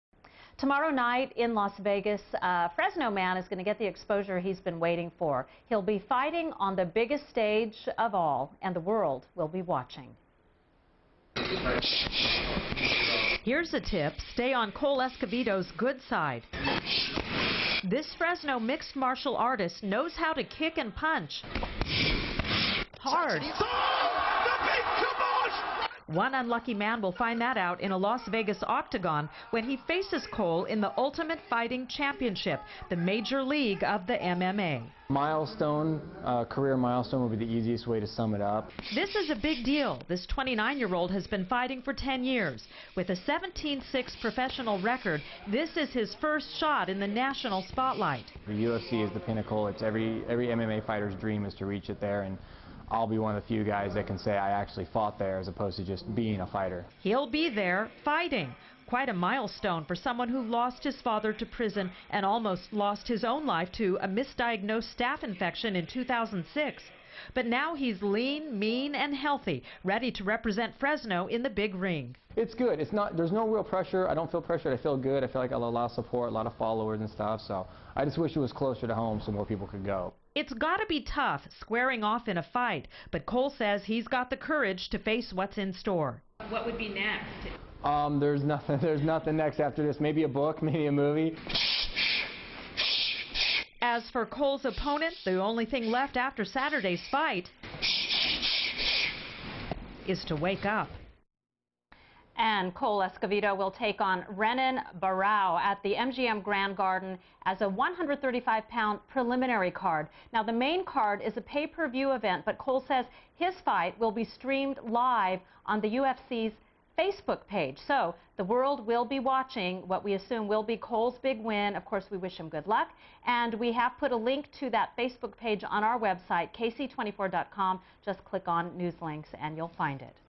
UFC shot for Fresno Fighter.
Fresno mixed martial artist Cole Escovedo has an opportunity to fight on the biggest stage of all: in the octagon for the Ultimate Fighting Championship. Stefani Booroojian has the story...